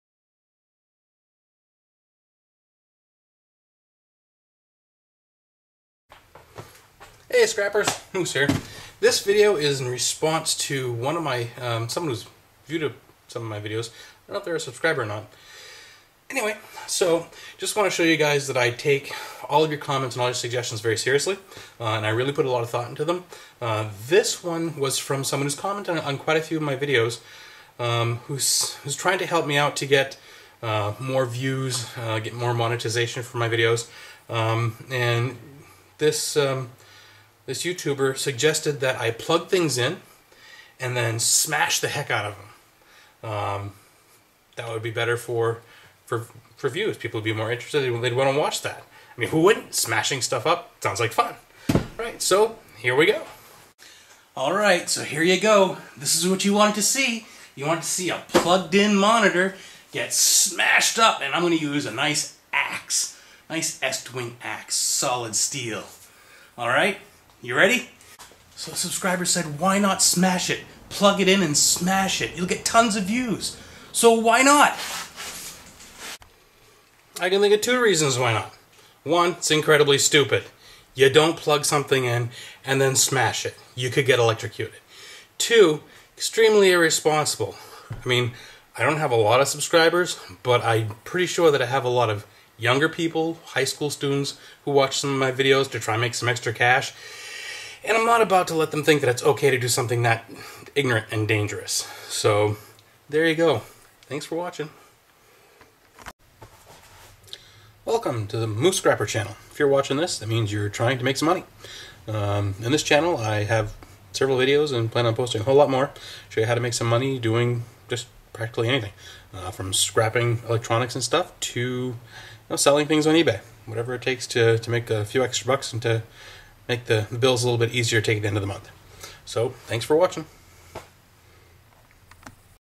How to scrap a plugged in CRT monitor by smashing it to bits with an axe
Here is a video response to one of my viewers trying to help me to get more youtube views.  I take all comments and suggestions seriously!!!